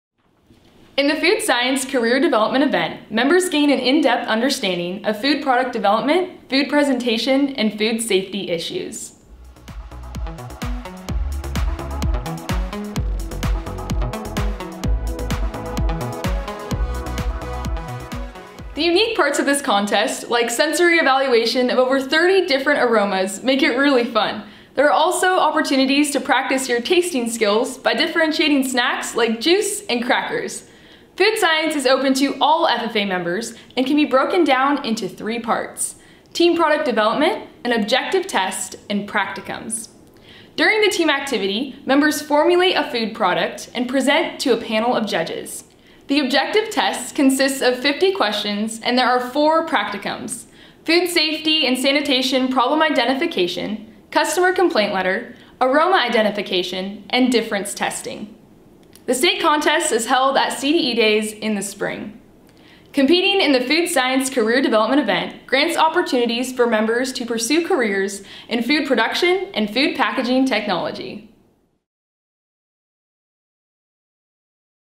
CDE Spotlight: Food Science
Students competing in the Food Science and Technology CDE display an in-depth understanding of food product development, food presentation, and food safety issues.